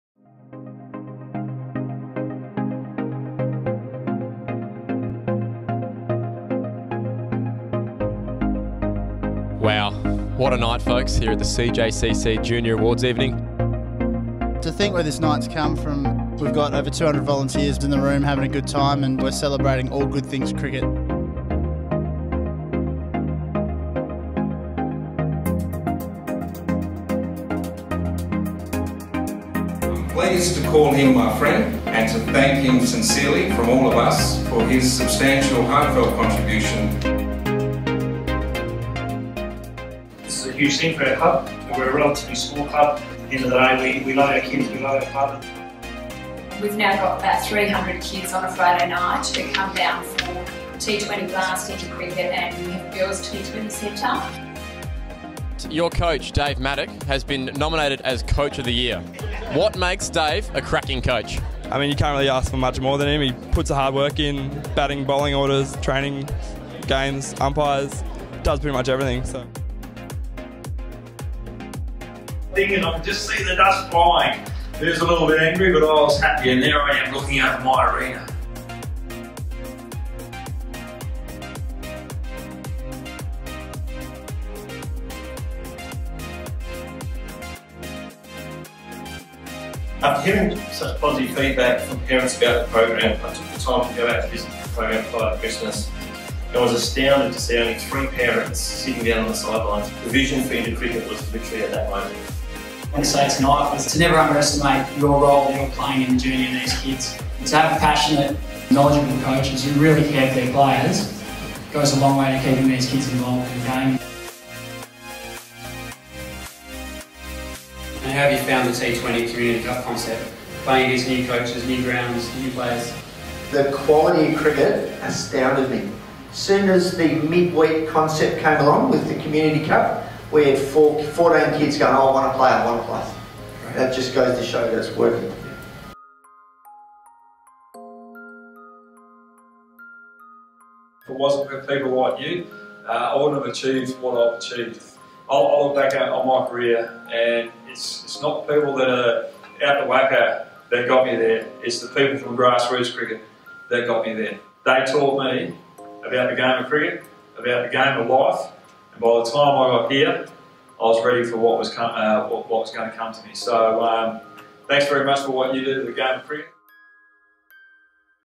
Community Junior Cricket Awards Night 2014-15
Community junior cricket was celebrated with approximately 230 people attending the annual awards evening at the Boundary Room at the WACA Ground